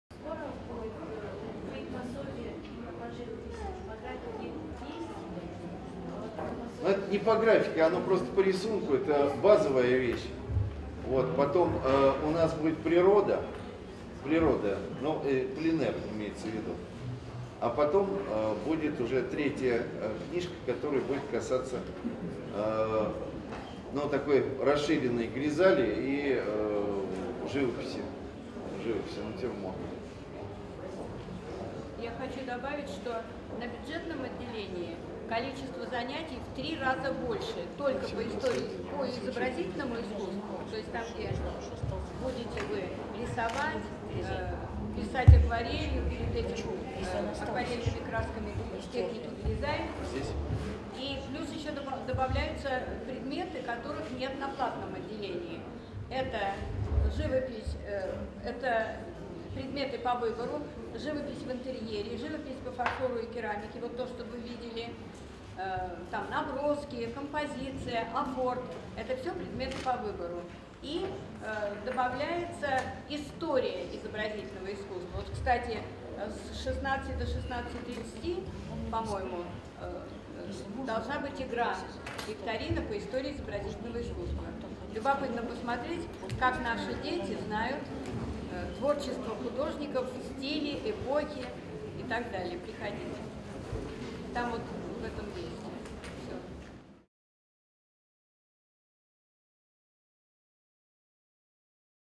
С. Н. Андрияка отвечает на вопросы гостей ШКОЛЫ АКВАРЕЛИ. 10/16
ВСТРЕЧА ГОСТЕЙ ШКОЛЫ АКВАРЕЛИ С ХУДОЖЕСТВЕННЫМ РУКОВОДИТЕЛЕМ С.Н. АНДРИЯКОЙ И ПЕДАГОГИЧЕСКИМ КОЛЛЕКТИВОМ.